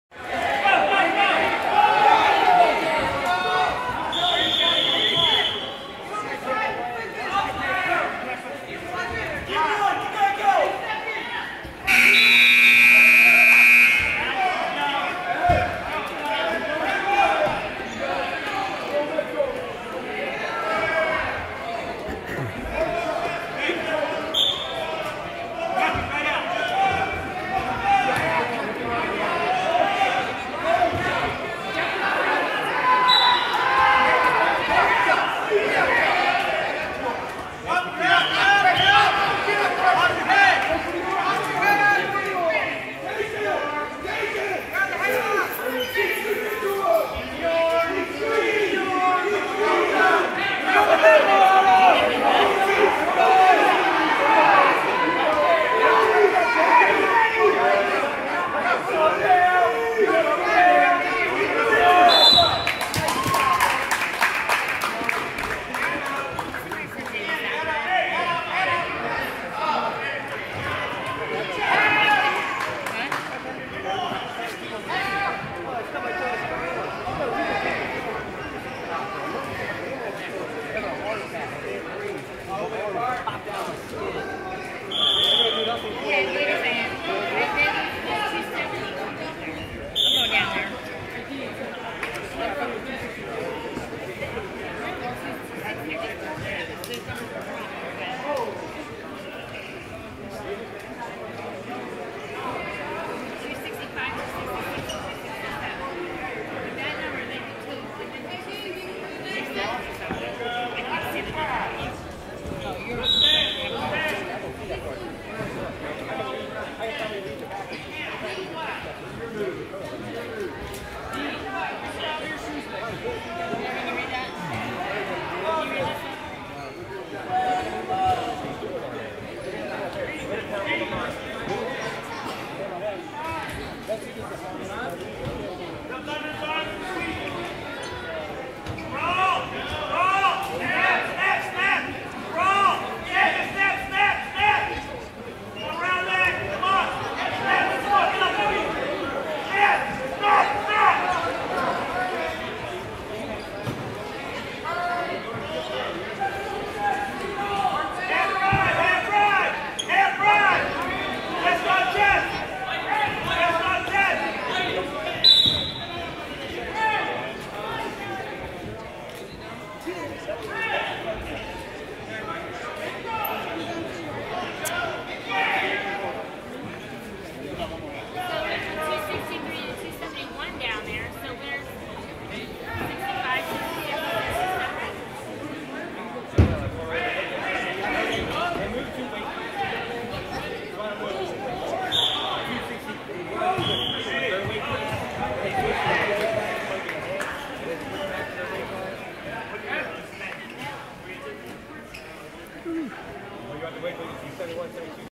La Plata HS 152lbs (Alex Strong) pins Chopticon HS
2015 HS SMAC wrestling tournament